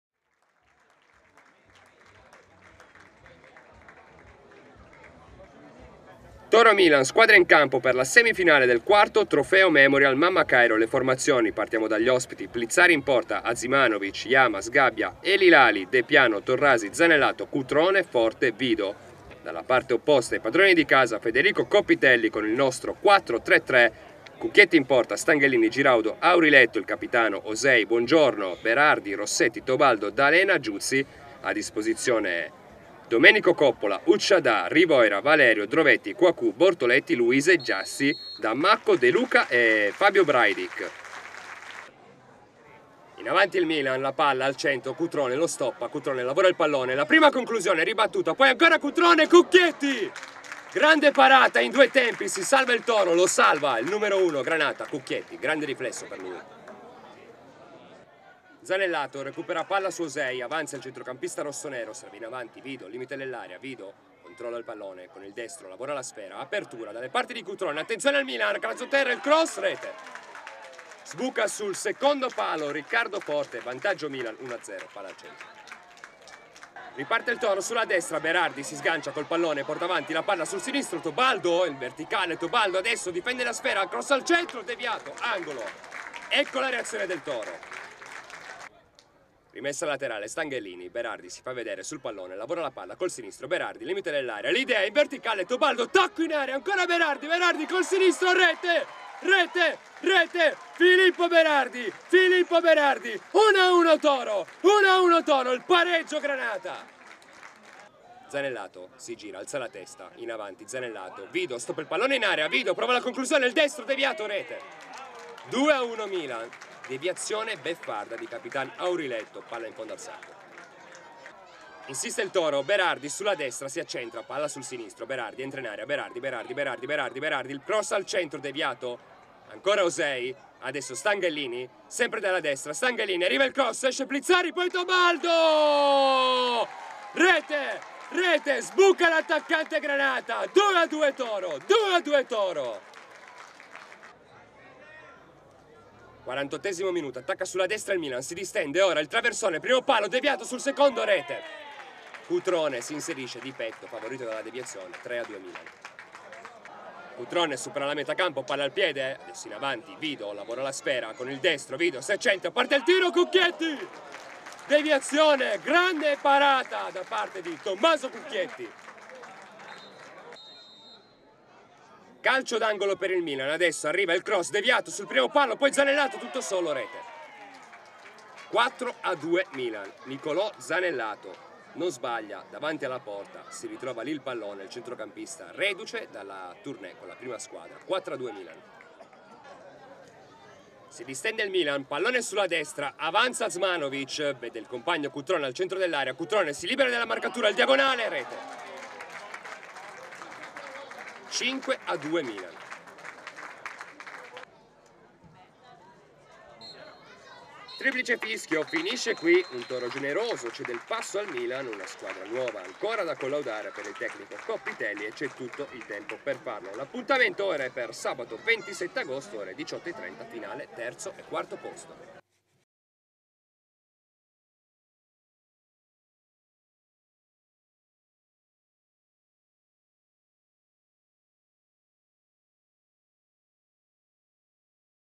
Trofeo "Mamma Cairo" - Torino-Milan 2-5: i gol
4° Memorial: gli highlights del match